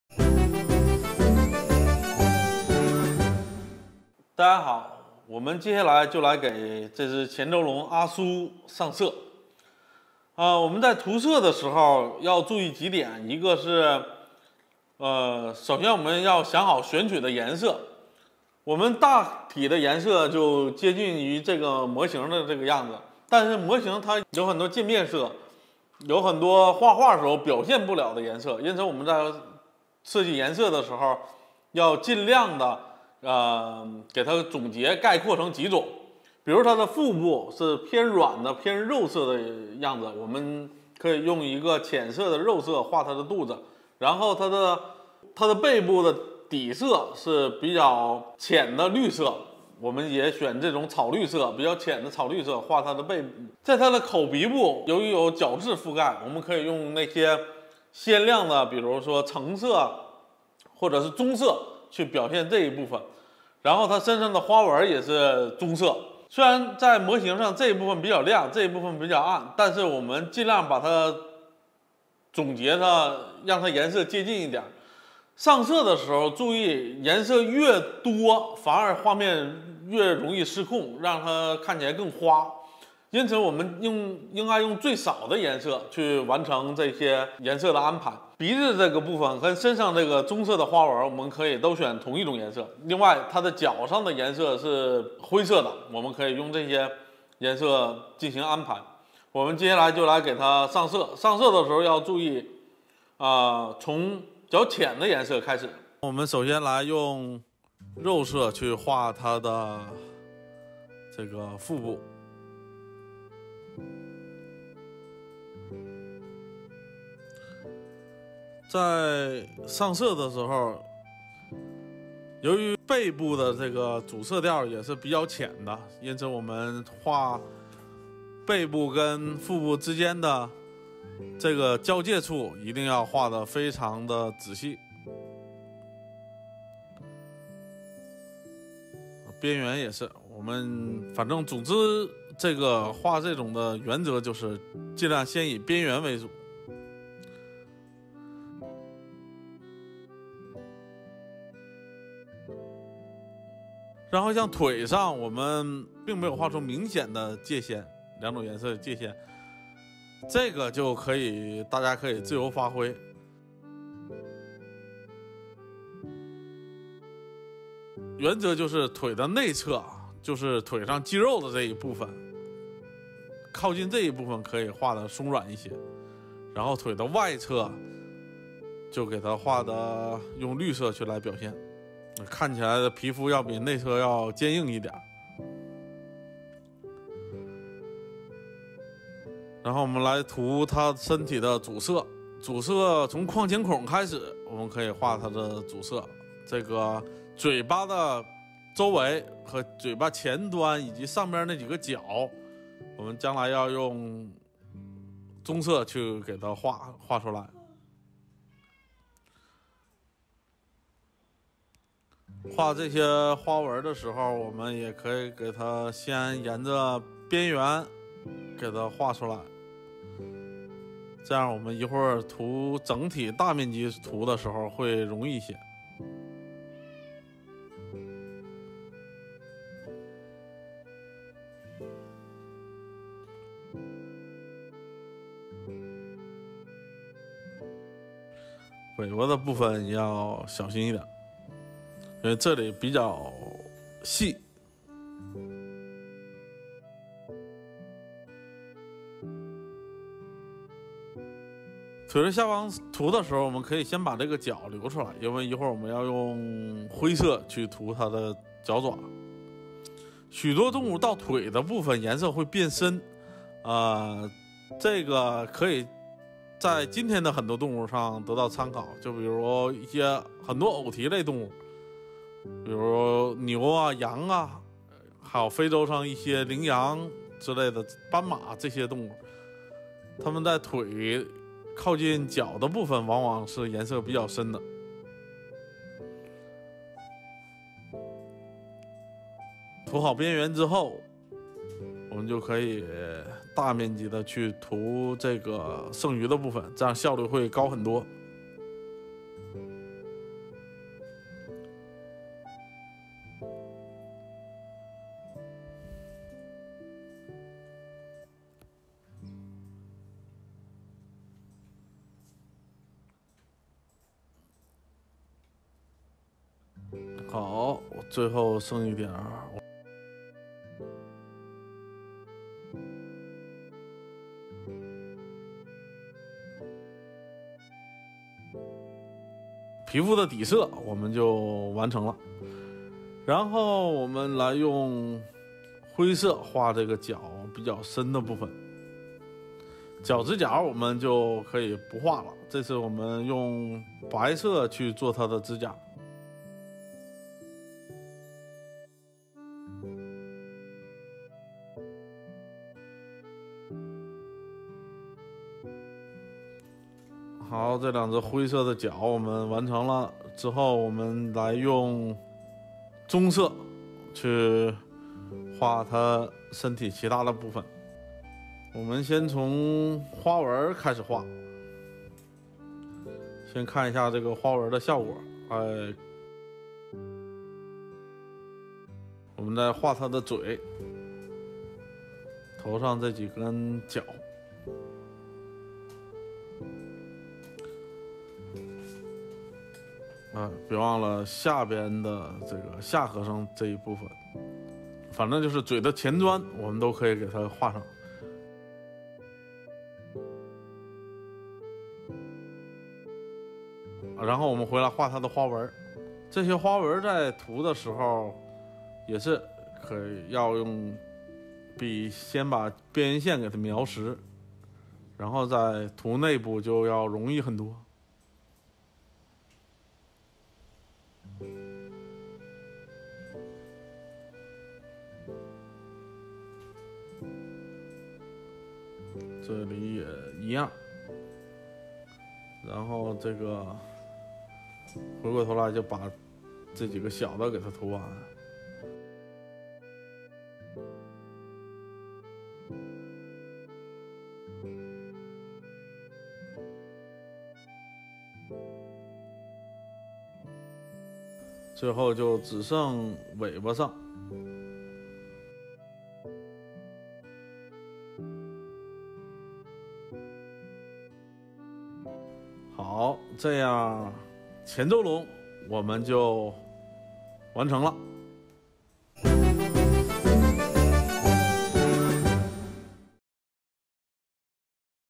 Qianzhousaurus 8: The Color | Learn to Draw Dinosaurs with ZHAO Chuang
Key Point
Coloring on paper is different from coloring a model, because the former has less scope in presenting gradient colors. Using more colors make it more likely that the picture may become chaotic. We have to use as few colors as possible to show the different textures of Qianzhousaurus.
Online Stores:
Amazon (US)